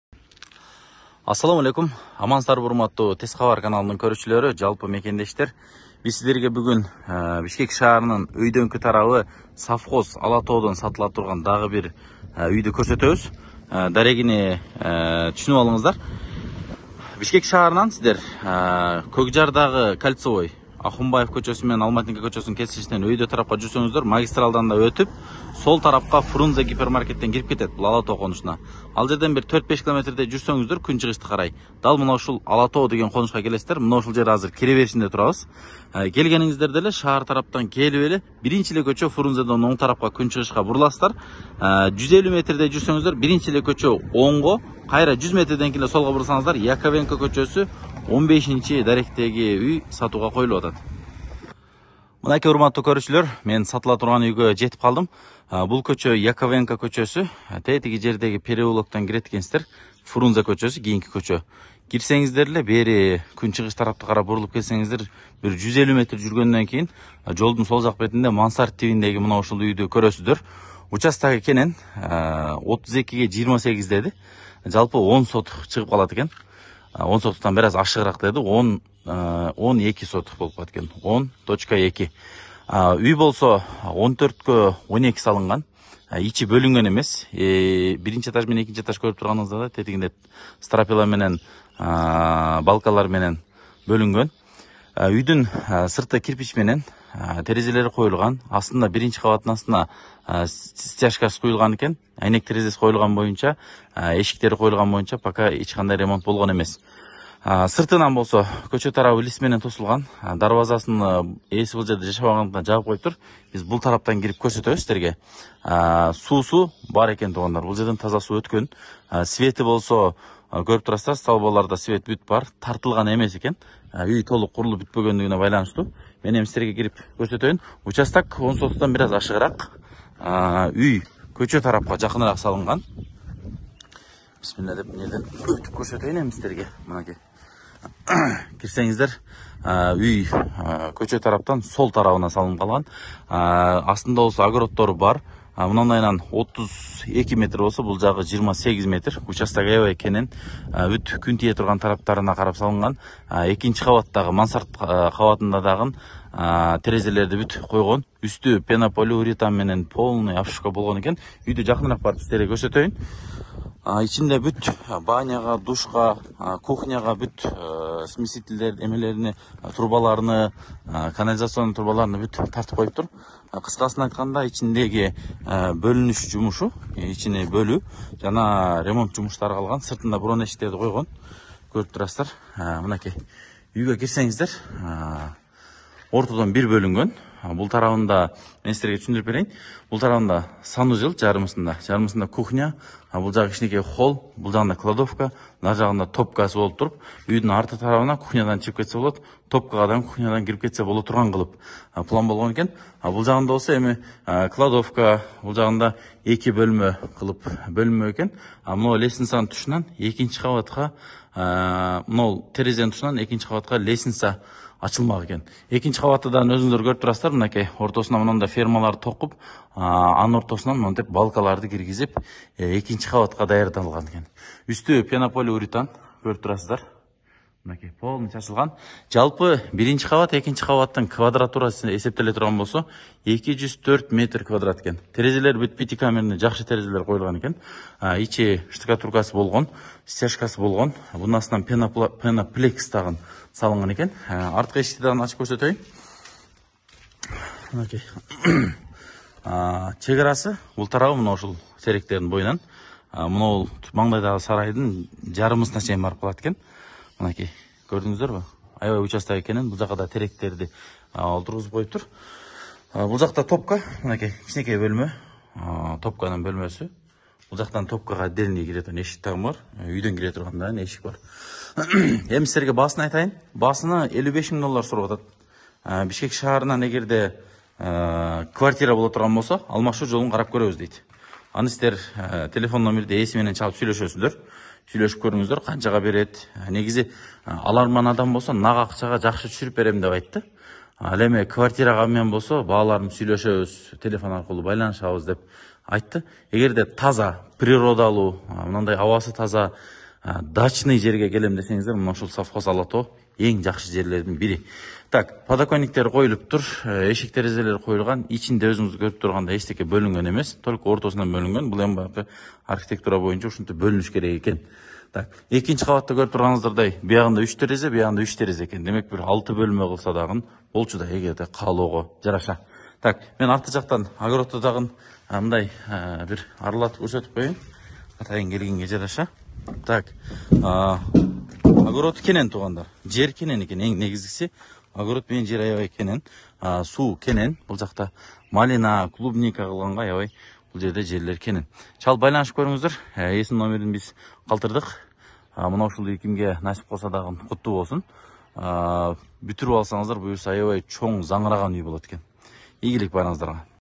Жаӊы курулган МАНСАРД тибиндеги ҮЙ сатылат ~ Жери 10,2 сотик , Документи так
Бишкек шаарынын жогорку тарабы Савхоз Ала-тоодон үй сатылат
Уч 10,2сотых
Үй 14х12
Баасы 55000$